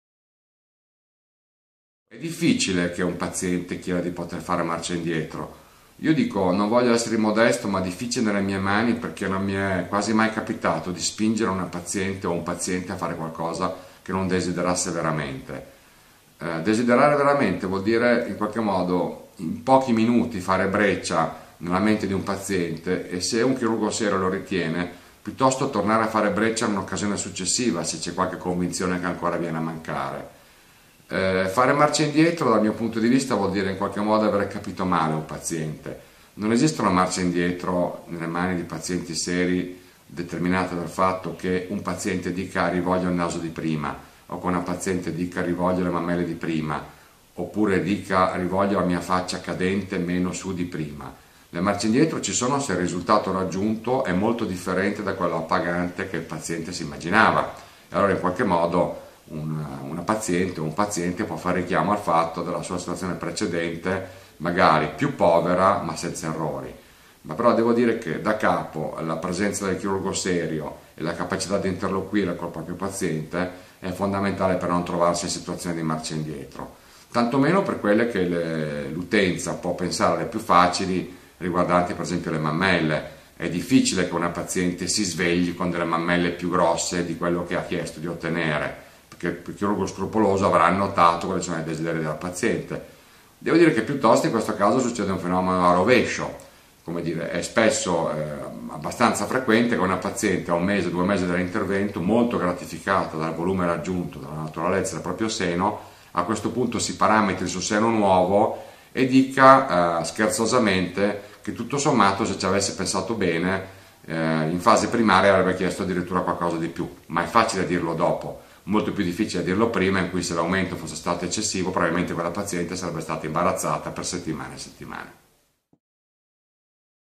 Ripensamenti dopo un intervento estetico?
Possono capitare ripensamenti dopo aver affrontato un intervento estetico? Come comportarsi? Ce lo spiega il Prof. Marco Klinger